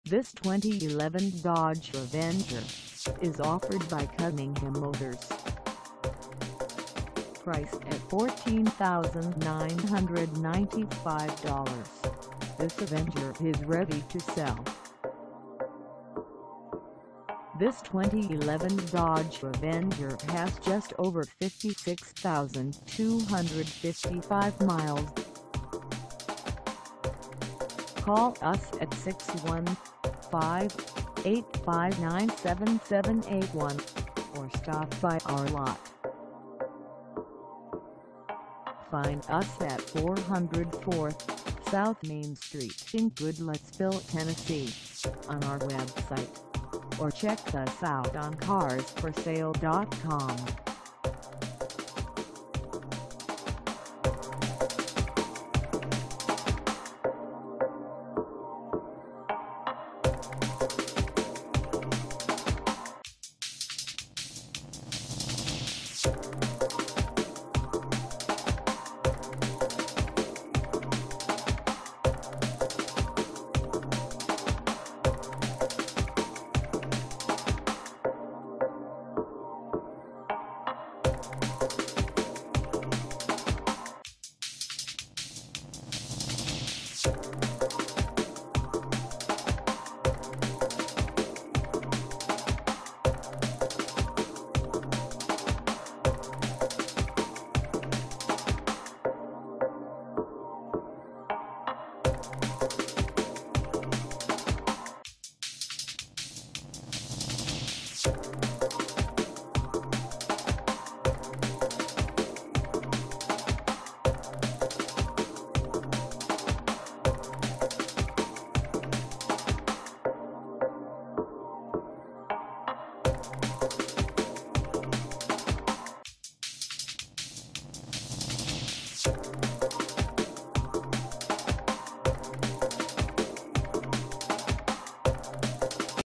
2011 Dodge Avenger Lux 4dr Sedan for sale in Goodlettsville,
This 2011 Dodge Avenger Lux 4dr Sedan is for sale in Goodlettsville, TN 37072 at Cunningham Motors .
Engine: I4 2.4L I4
Transmission: Automatic 6-Speed
Color: Red
Mileage: 56255
Contact Cunningham Motors  404 South Main Street  Goodlettsville, TN 37072 to test drive this 2011 Dodge Avenger Lux 4dr Sedan today.